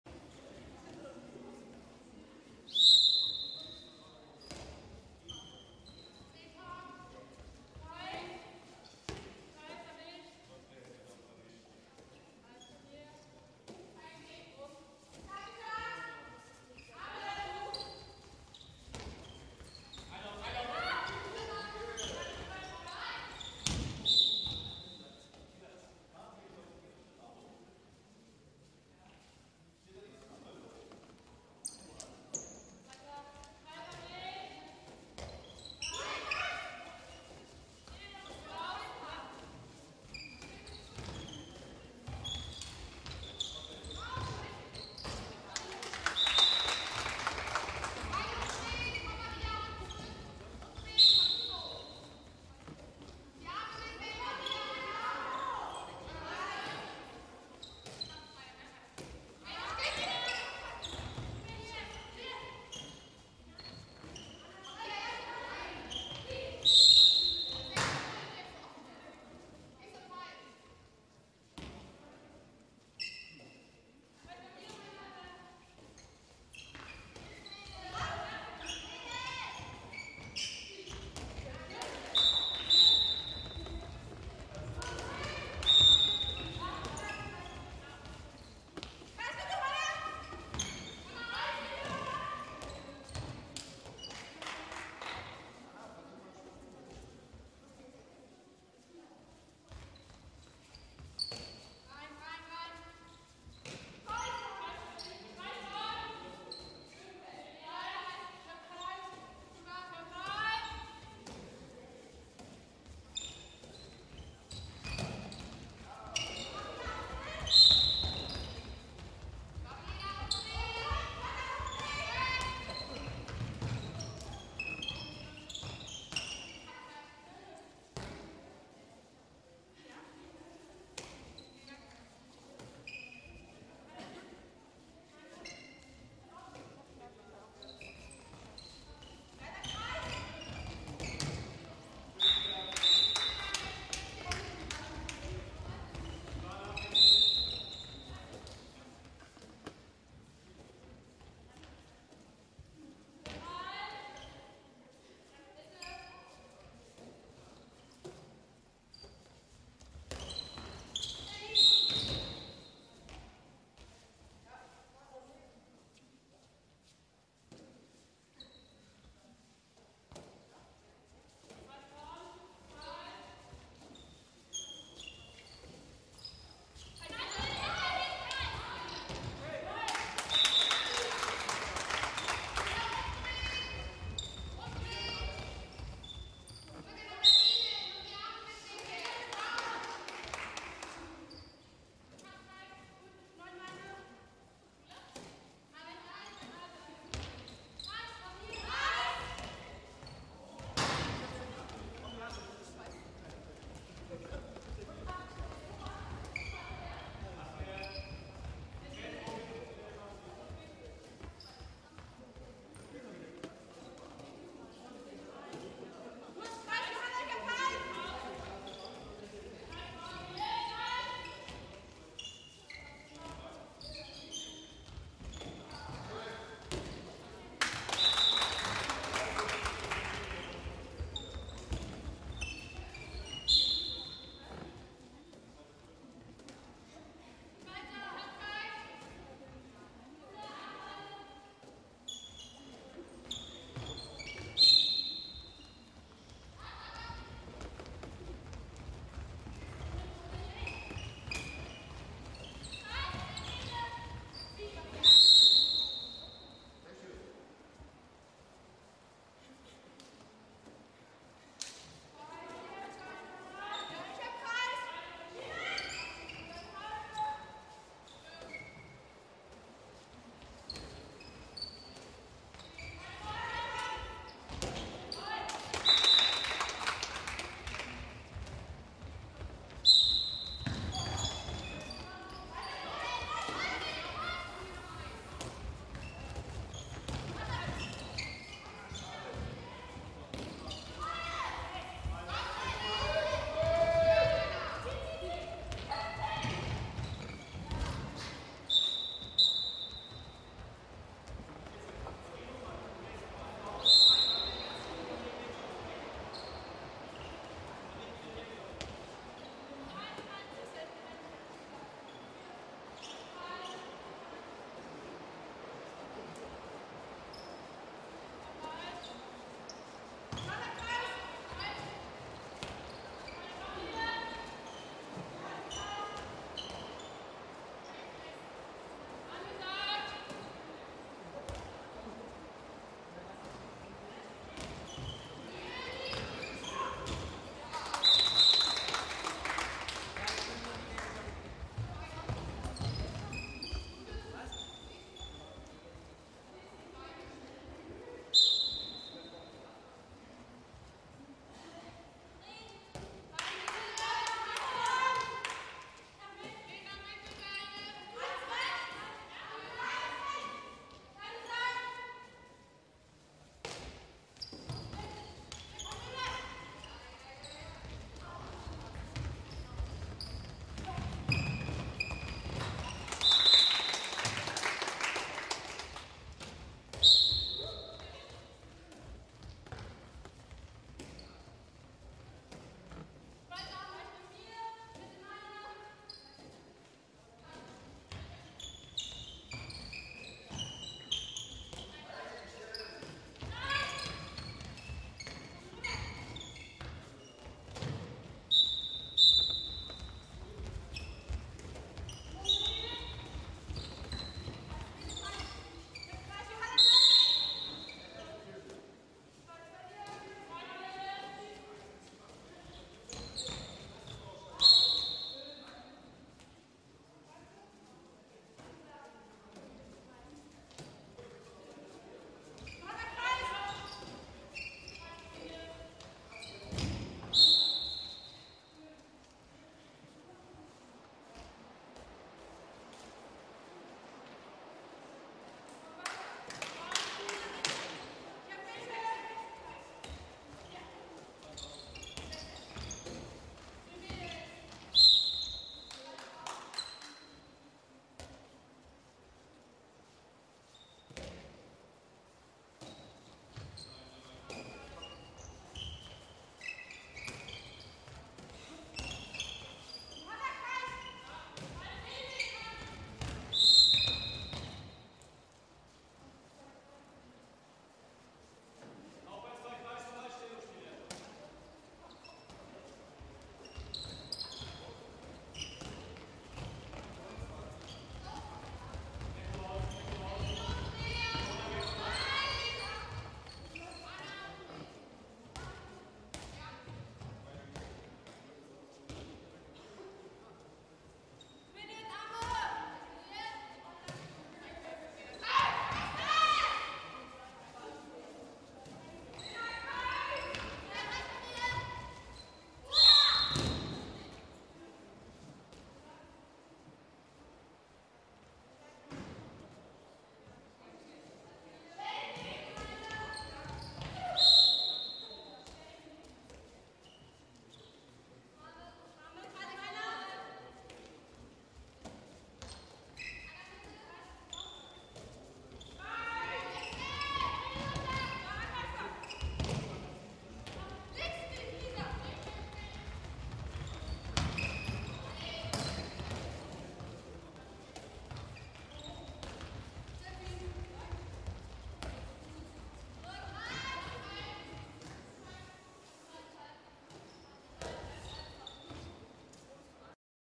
Sport inSchwerin 20s2 T 06
Handball weibliche A Jugend Sporthalle Kästnerschule, Schwerin Meckl. SV Grün Weiß vs. Vfl Bad Schwartau